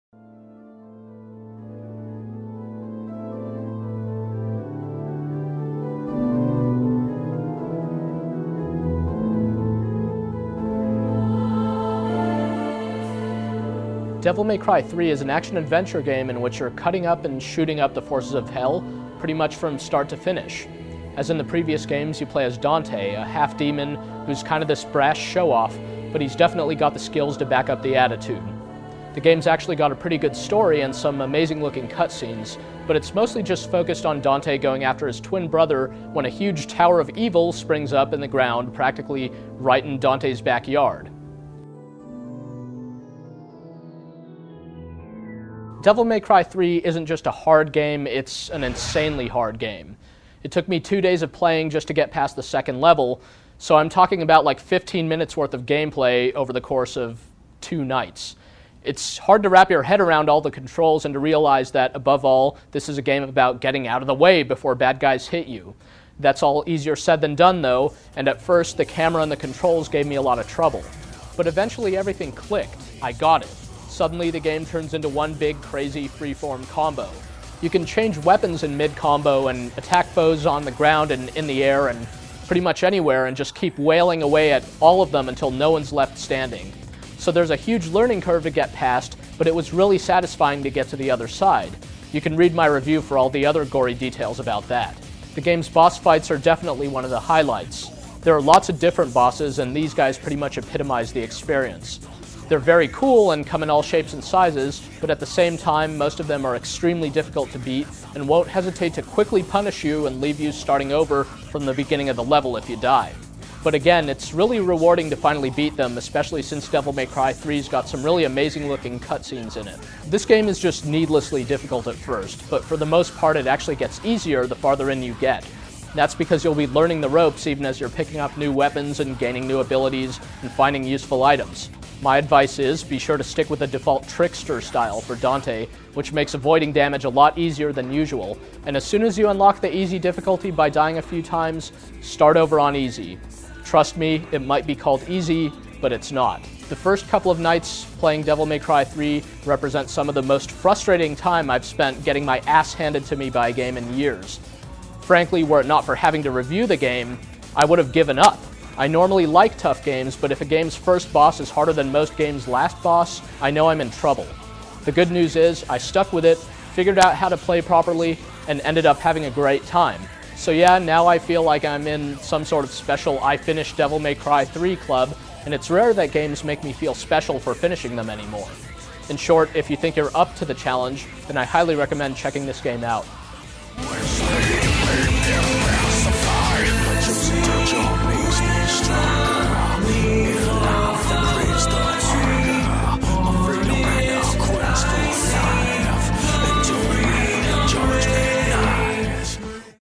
GameSpot - Devil May Cry 3: Dante's Awakening Video Review (PS2)
Greg Kasavin reviews DMC3 on PS2.